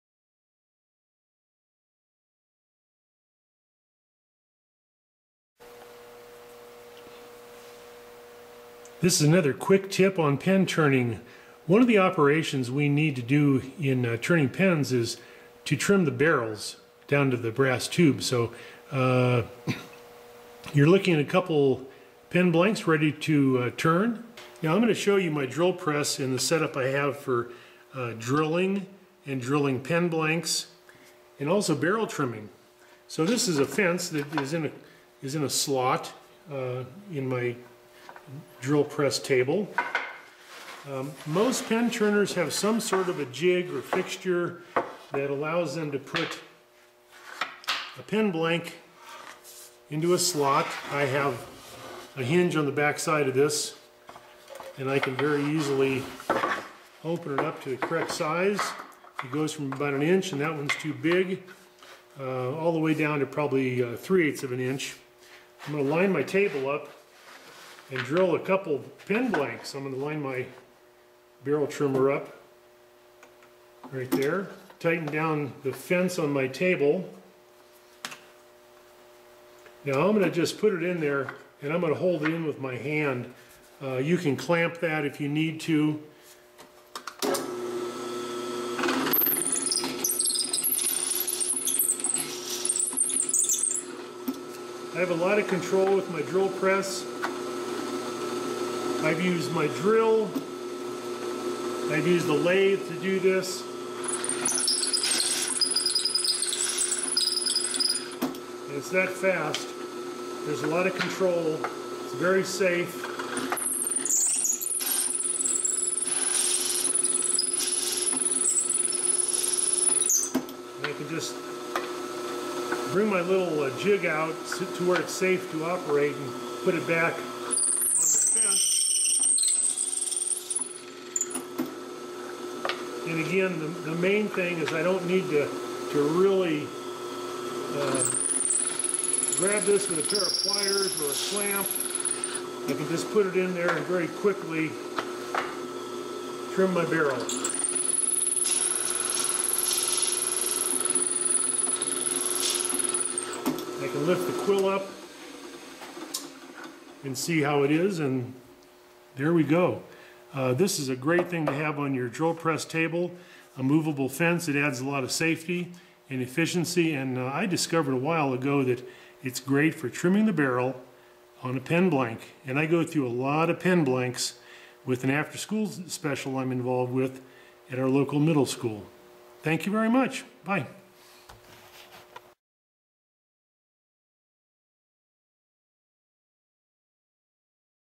PEN MAKING TIP: BARREL TRIMMING on the Drill Press
This short demonstration shows how to trim a pen blank barrel on the drill press